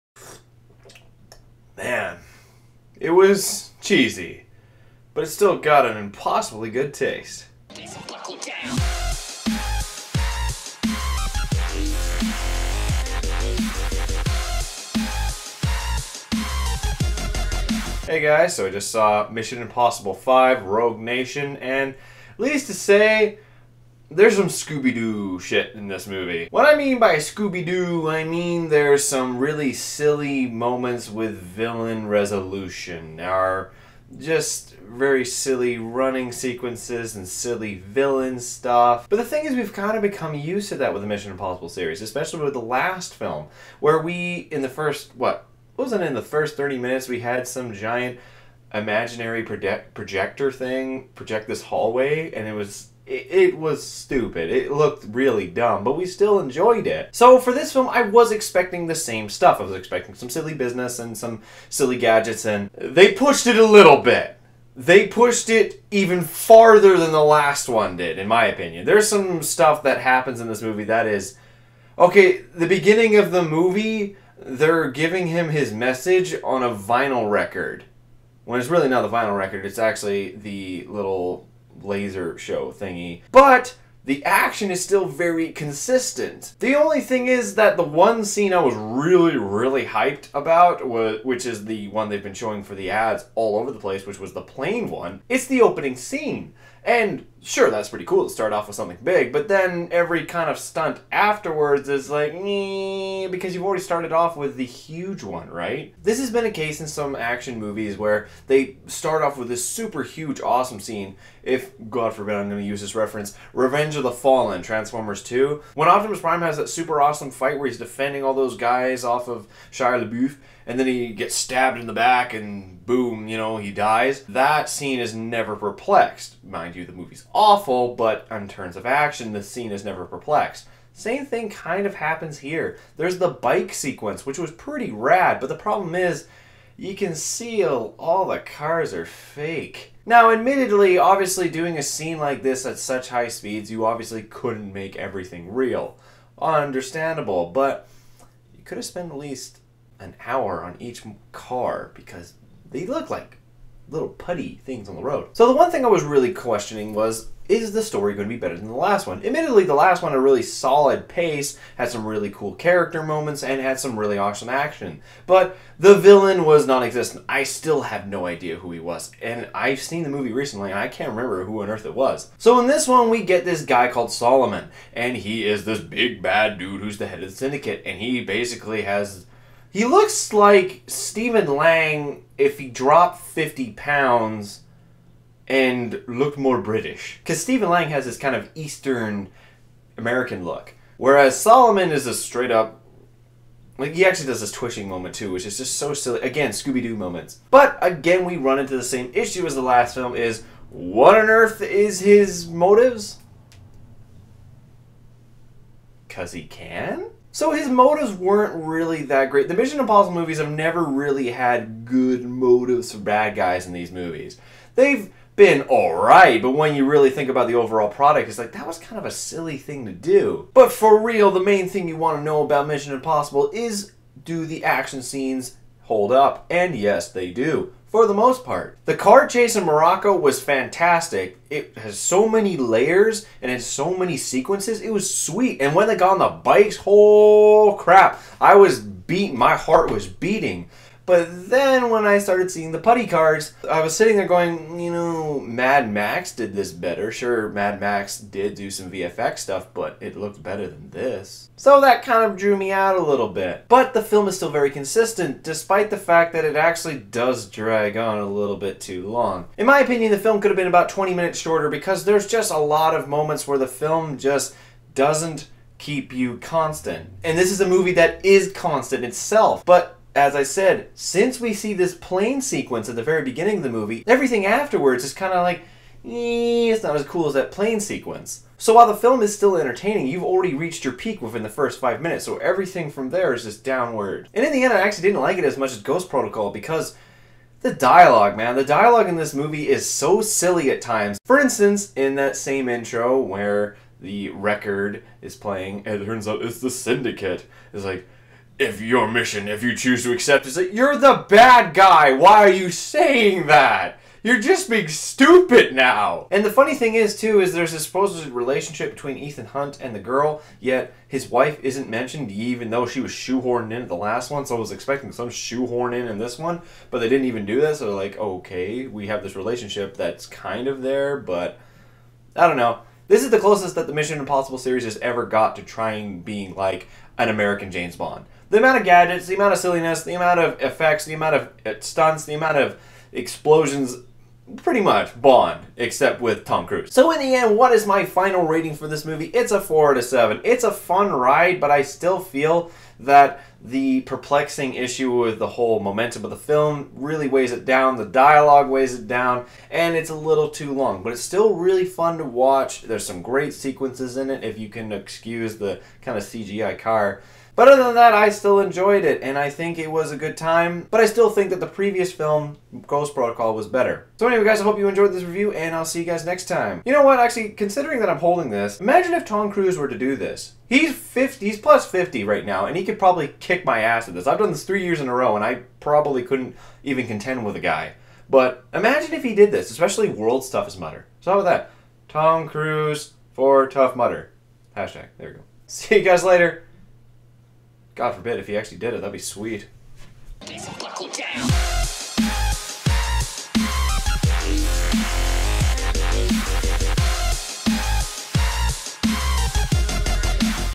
Mission Impossible Rogue Nation Movie Review
So how do the stunts and story add up in Tom Cruise's newest installment of the Mission Impossible series? Kind of like a Scooby Doo episode.